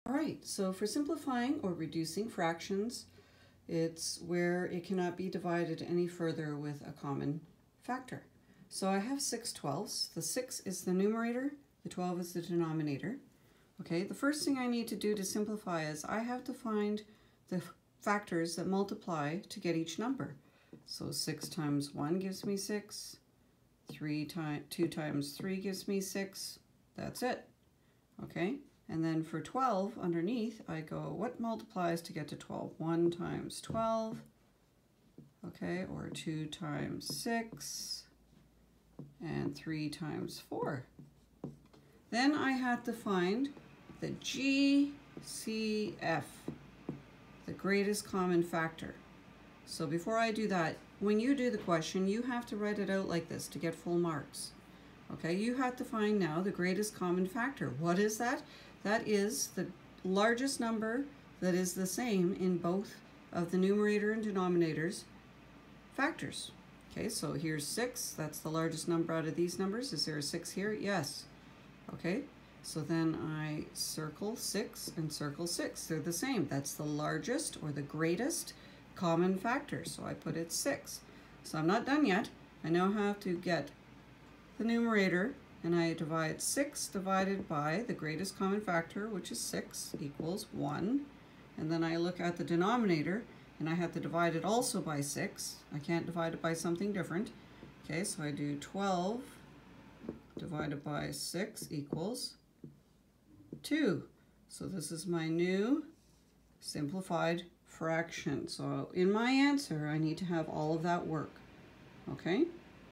Easy way to Simplify/reducing Fractions to lowest Form
A short video to show how you can simplify and reduce fractions to their lowest form.  Find the GCF!